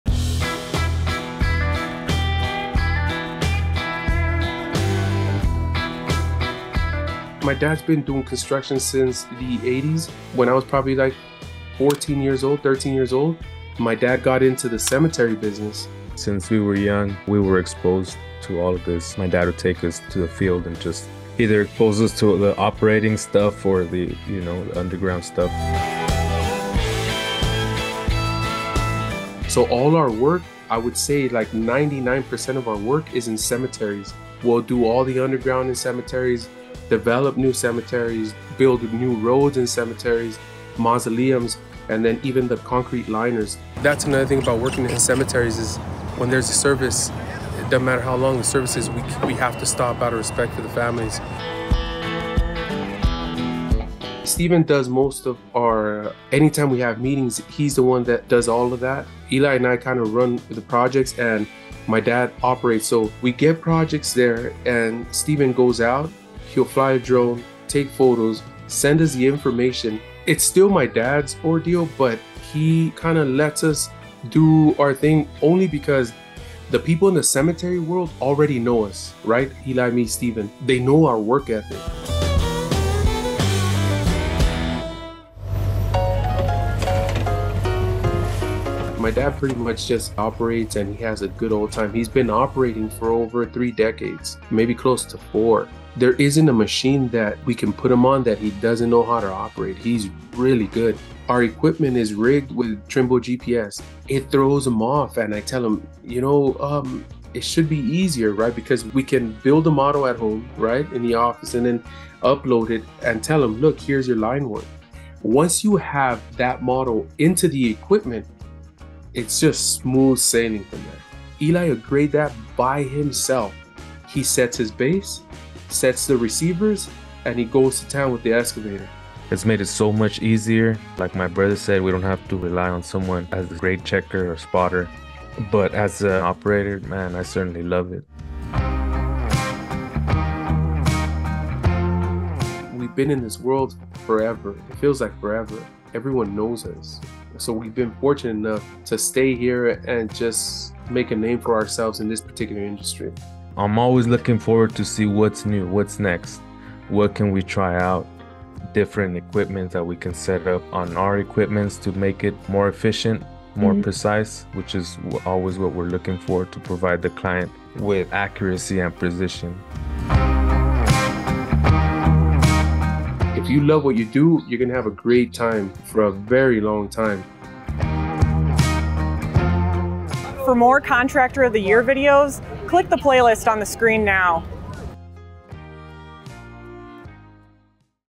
Brothers Make Machine Control Work on Precise Cemetery Jobs
Redlands, California-based Buenas Brothers Engineering, owned by Javier, Emilio, Eli and Steven Buenas, is one of 12 finalists for Equipment World’s 2024 Contractor of the Year Award.

In the video above, Javier and Eli Buenas talk about how their father, Emilio, established the foundation for the company’s stellar reputation in installing cemetery crypts and mausoleums.

With Emilio now concentrating on lending his expertise in operating equipment, brothers Javier, Eli and Steven have taken a leadership role in the company. And with the change in generations has come an emphasis on using machine control technology.

Buenas Brothers’ work requires precise layout and installation of side-by-side double-depth concrete lawn crypts, placed less than 2 inches from each other and conforming to undulating grade elevations. Exactness is required to not encroach on future crypt installations, an important revenue source for cemeteries.

“Once you have the model in the equipment, it’s just smooth sailing from there,” Javier says.

“It’s made it so much easier,” adds Eli. “We don’t have to rely on someone as the spot grade-checker or spotter. As an operator, I love it.”

“We’ve been in this work forever, and everyone knows us,” Javier says. “So we’ve been fortunate enough to stay here and just make a name for ourselves in this particular industry.”